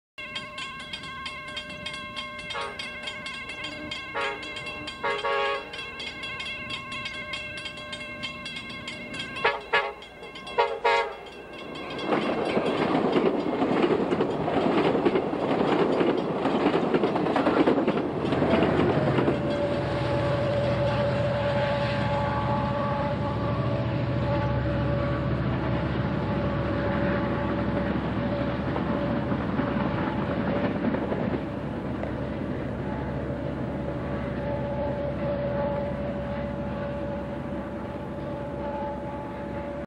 Amtrak California in Moorpark again... 1998
Another Amtrak California train in Moorpark in 1998. The superliner from the past you could say.